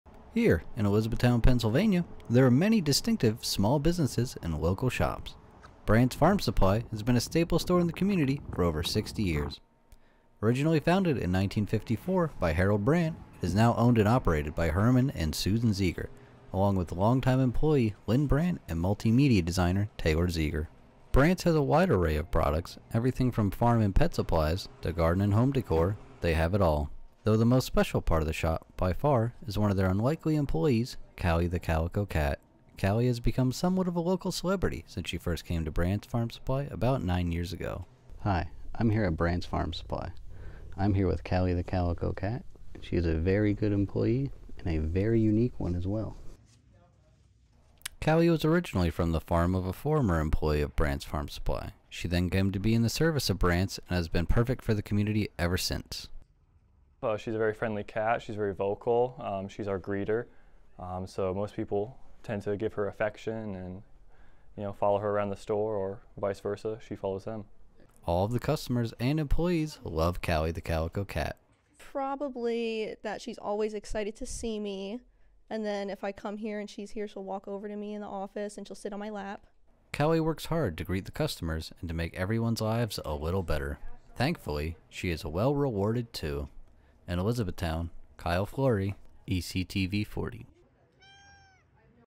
Friendly feline becomes customer favorite at local farm supply store
Elizabethtown is home to many unique small businesses, and every good business needs good employees. One particular farm supply store in Elizabethtown has a unique face working inside. ECTV-40's Kyle Florey went to see one part of what makes this shop so special.